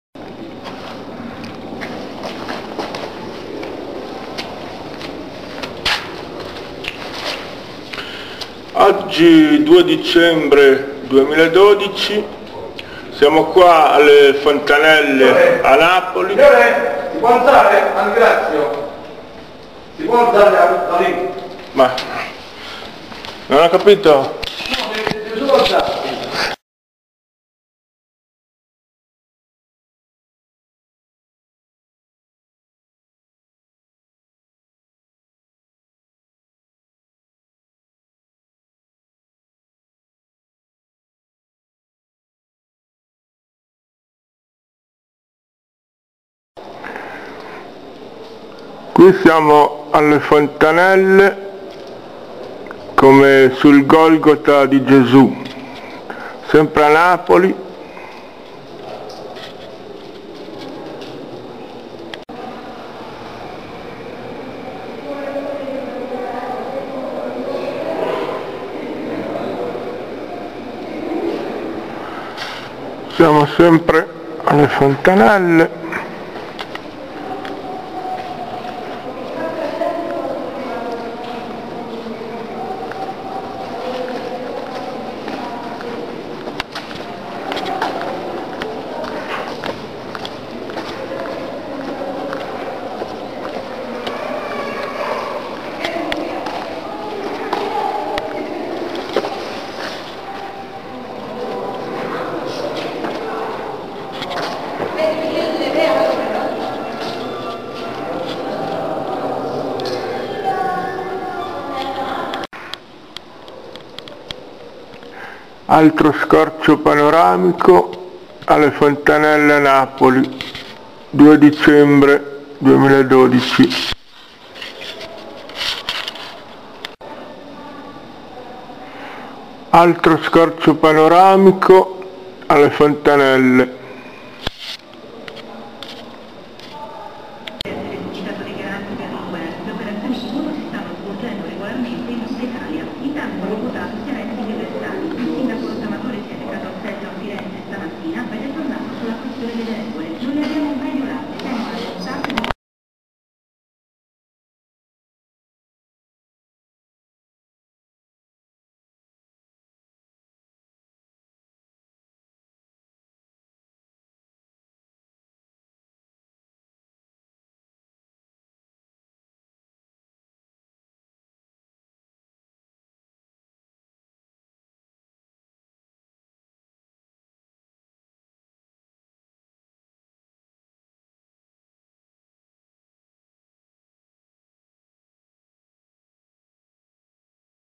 Video censura tra gli scheletri
Alle Fontanelle A Napoli, ciò che è possibile vedere.